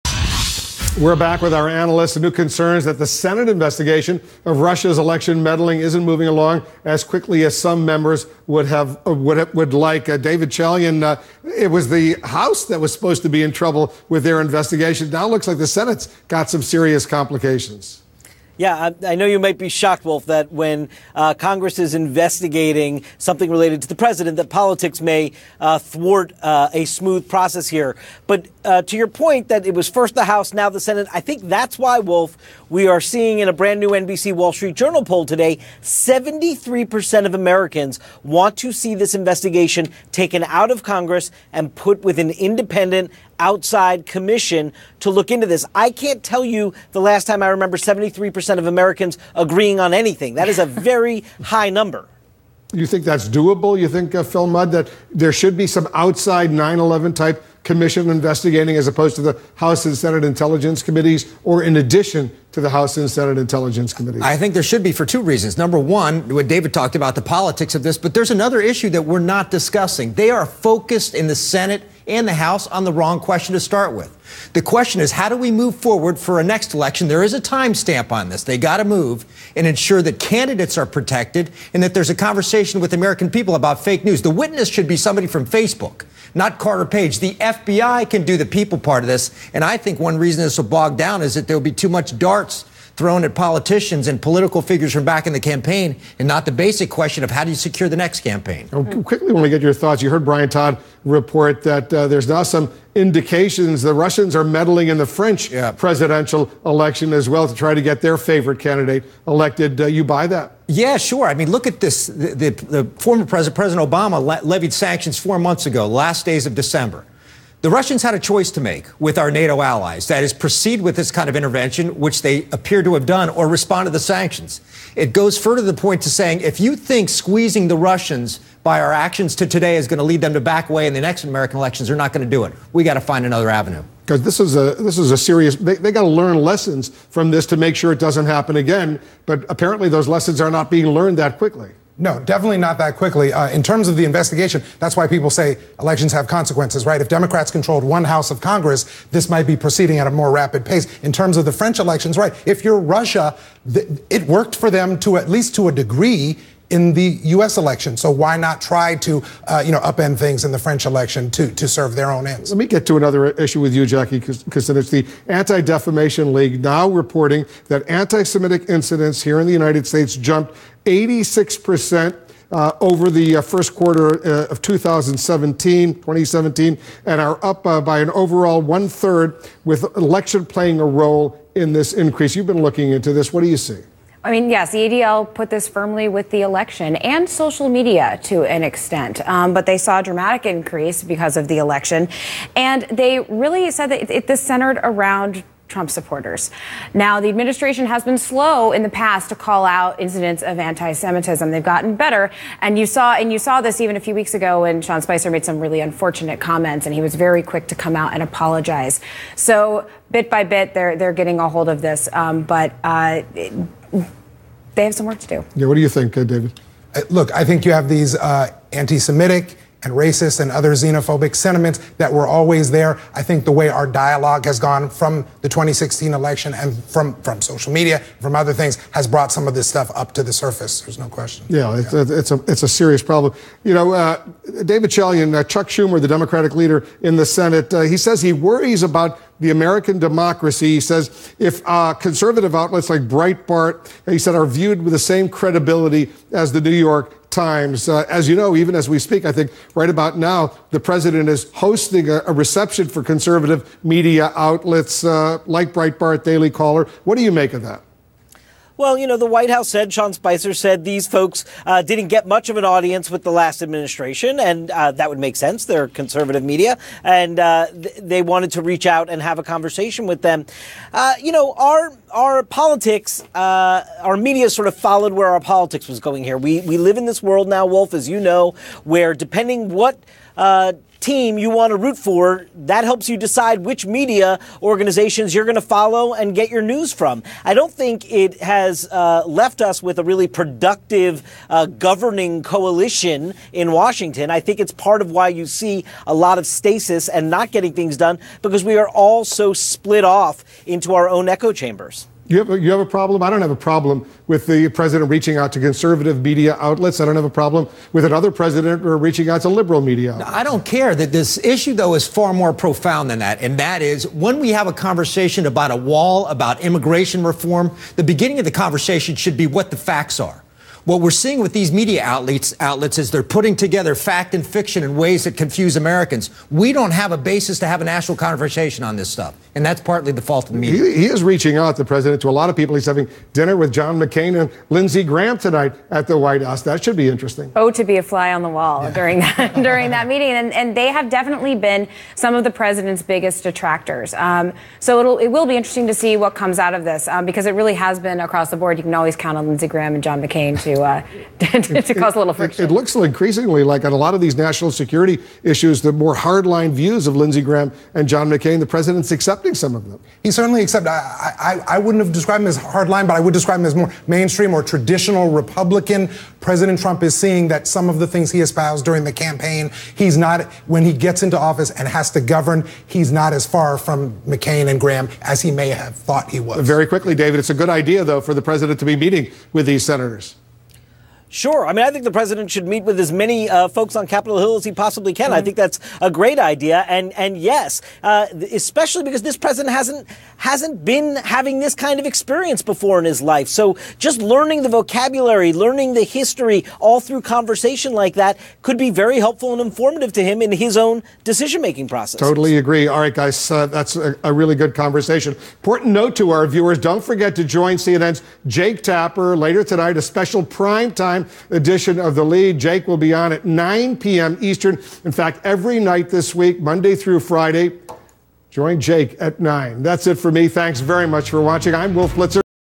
Experts: Signs Russia Trying to sway another pivotal Election.
The First 100 Days of President Donald Trump.